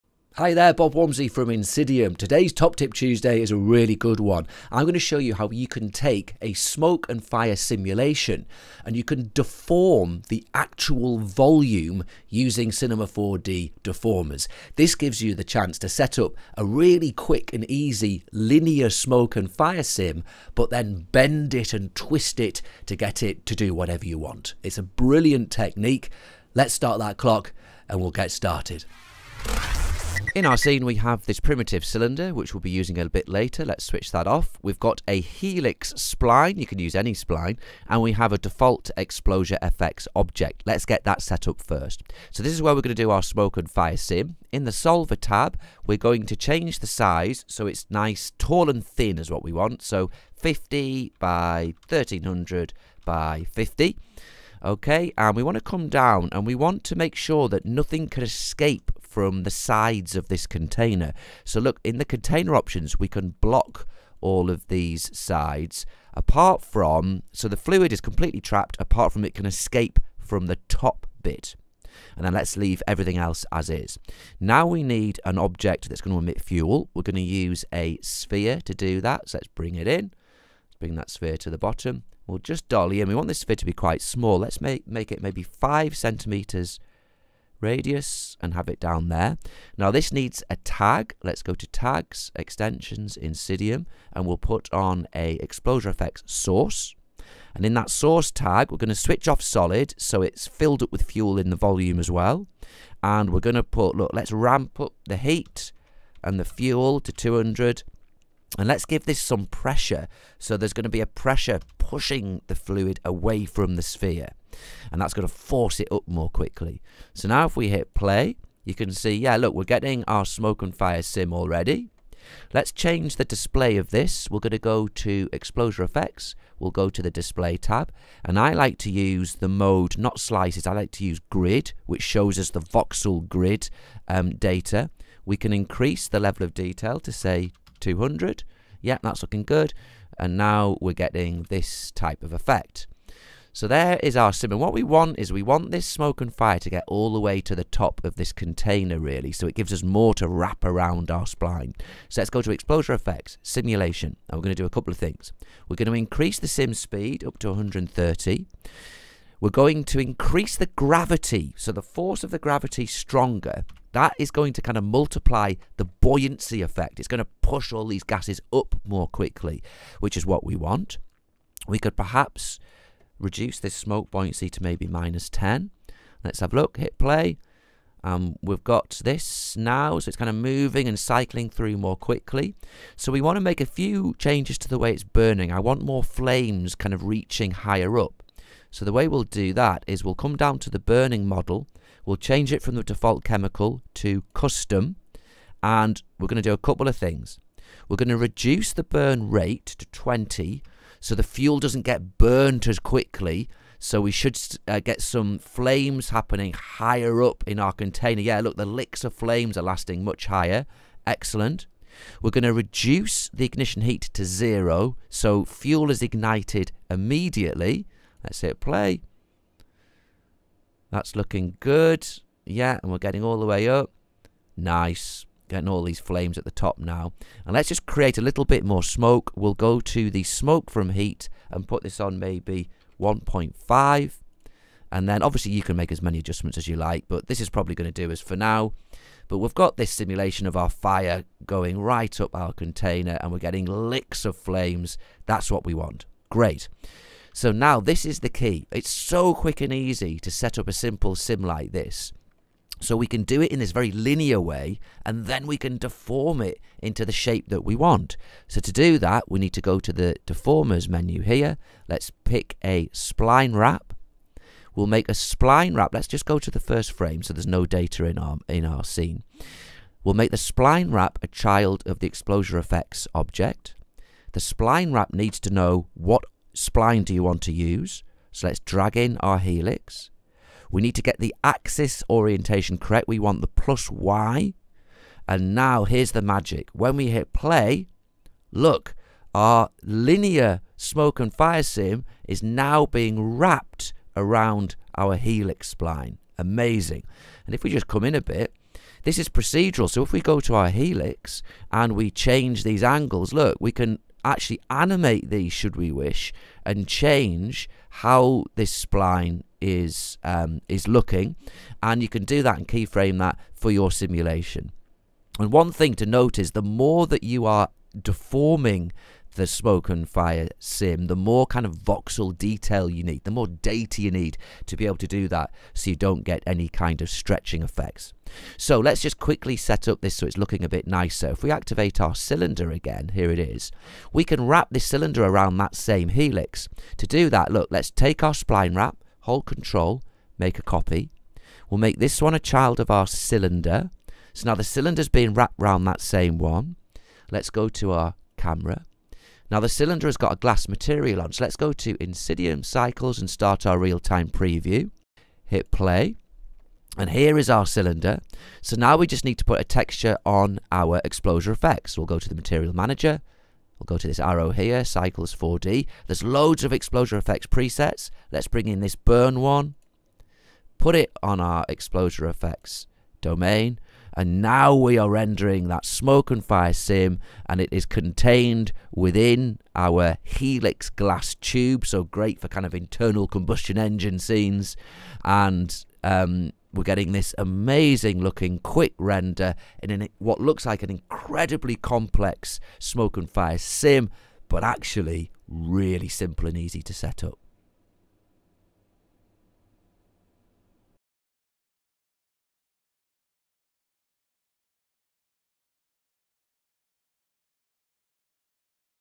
Top Tip Tuesday - Deforming Smoke and Fire
Today, in Top Tip Tuesday, we set up a simple, linear smoke and fire simulation, using xpExplosiaFX. We then make adjustments to the simulation and burning model settings to get the correct look before using a Spline wrap to deform the smoke and fire volume along the path of a Helix spline.